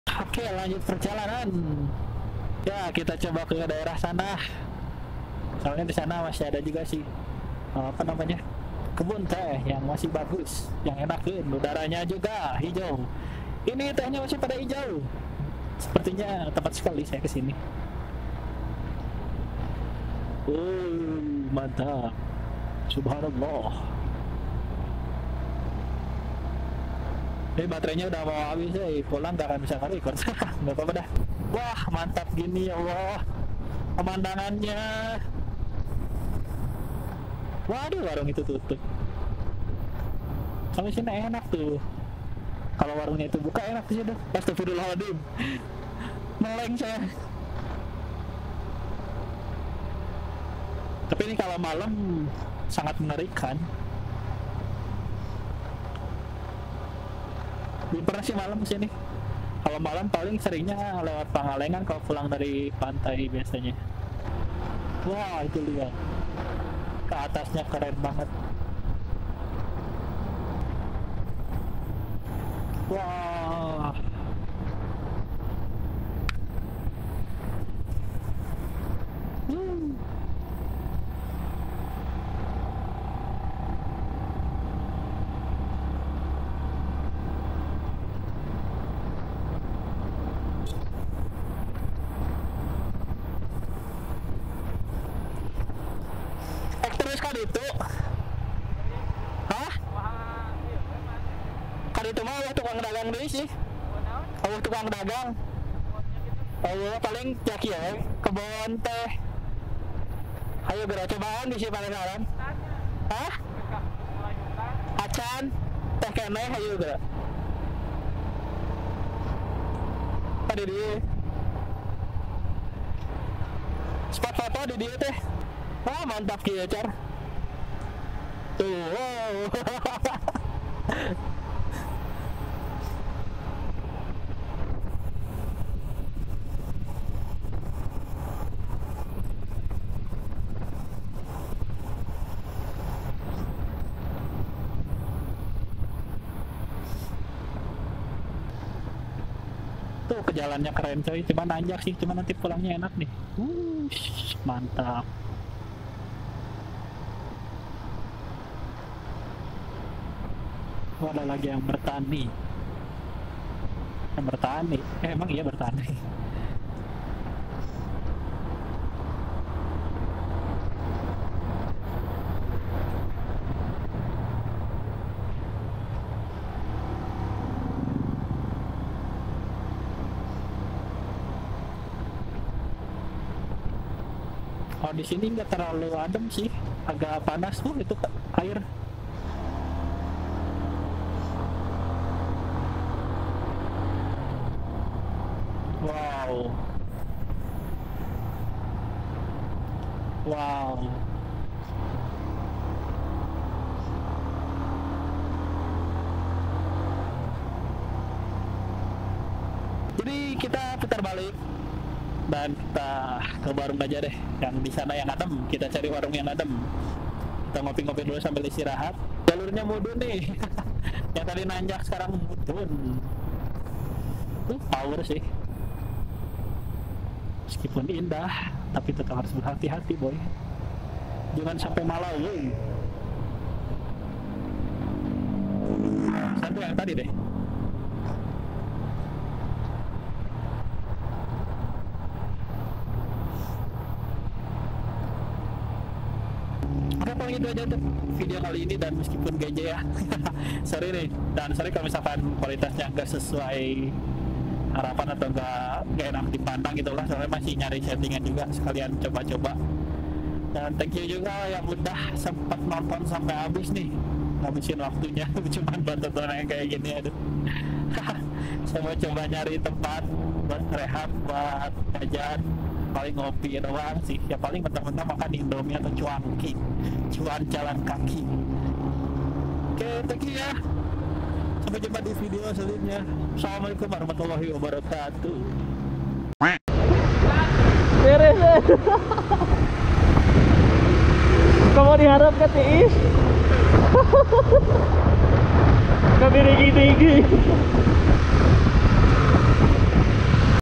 [Motovlog] INDAH DAN SEJUK, RECOMMENDED BUAT HEALING !! - Road to Ciwidey (MonMoRi) #2
Sorry kualitas audionya kurang enak di denger, setting mic nya terlalu deket sama bibir